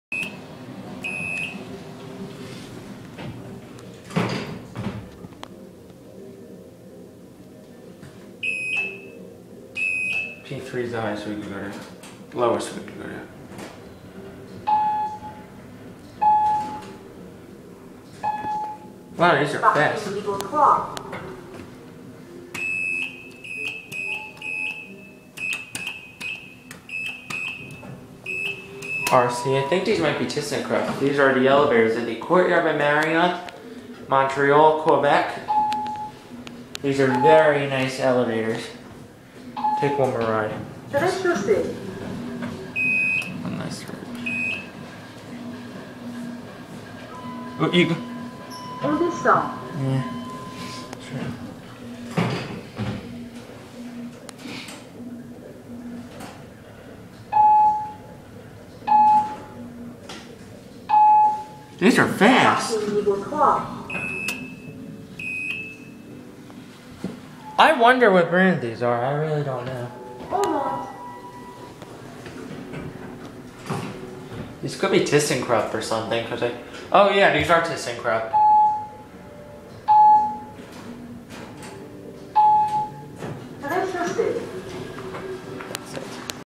Thyssenkrupp Traction Elevators At The Courtyard By Marriott In Montreal QC
These are the very nice Thyssenkrupp traction elevators at the Courtyard By Marriott in Montreal QC.